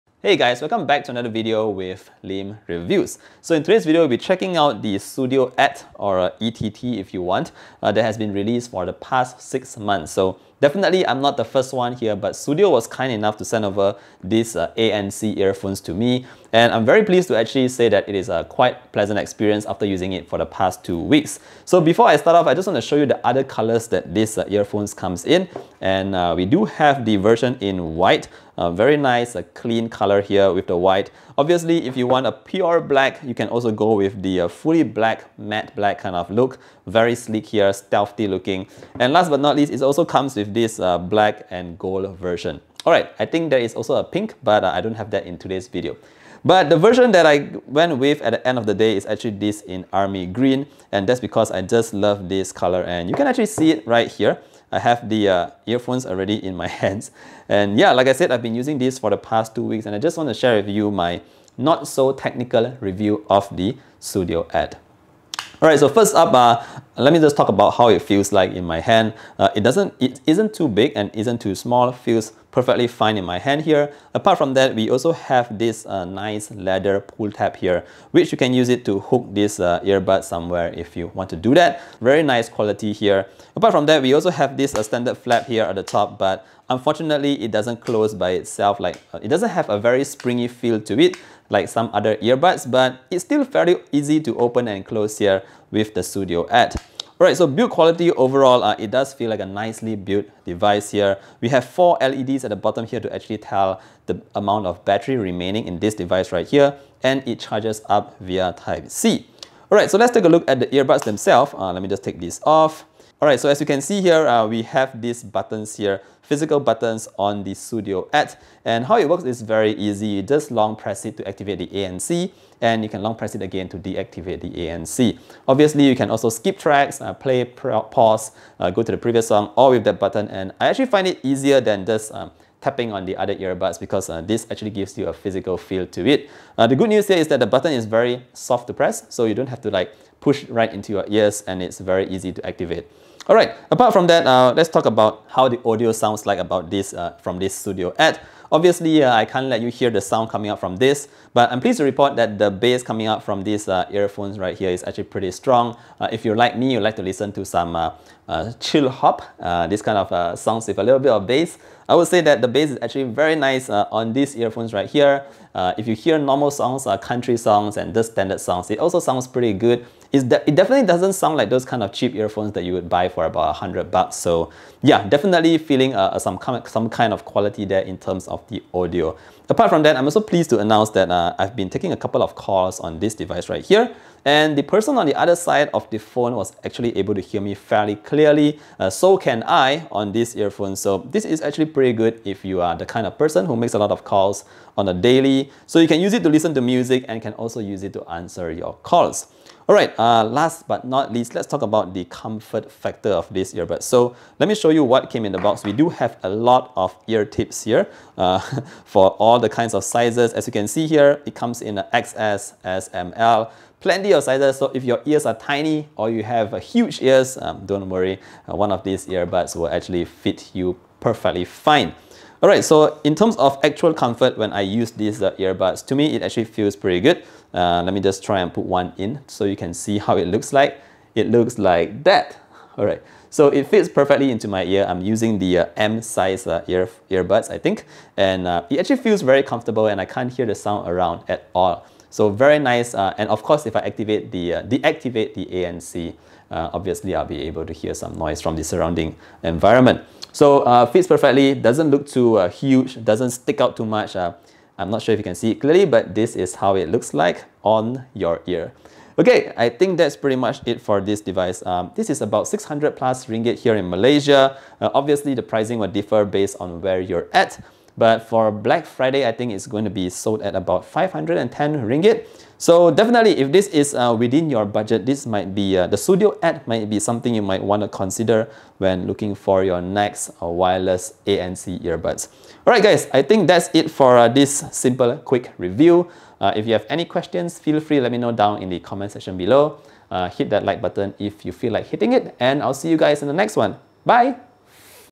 Sudio ETT ANC Earbuds: I LOVE THIS COLOR! And YES This Sounds Awesome!
OK so yes - Sudio sent this over to me to check out, and YES I absolutely love the color of the Sudio ETT ANC wireless earbuds hahaha. Watch the video to find out how someone, who isn't exactly a massive audiophile like myself, think about using this on the daily. Cheers!